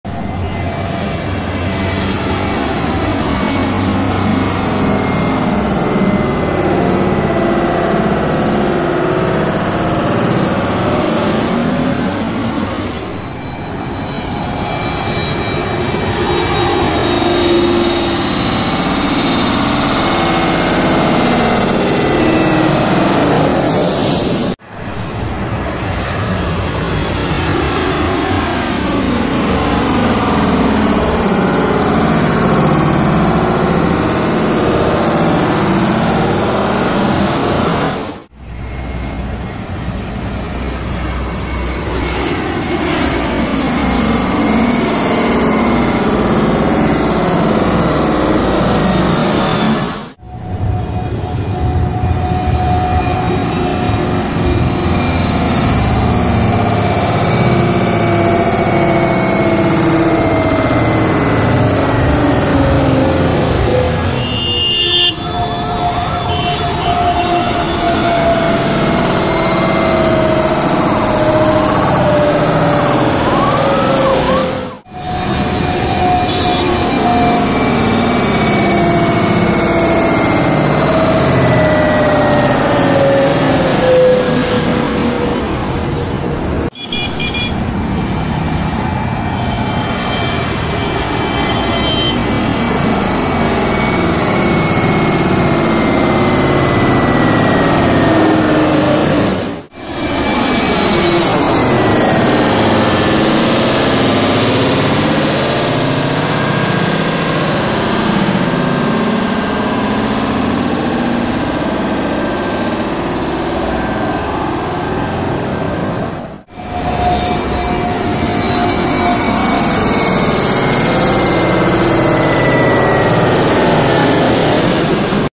Back to back landing at delhi airport | igi airport delhi | plane spotting
Hello everyone,
Plane spotting has always been full of joy for me that I love to share with all of those who are Aviation lovers. So here is the another plane spotting for all of you.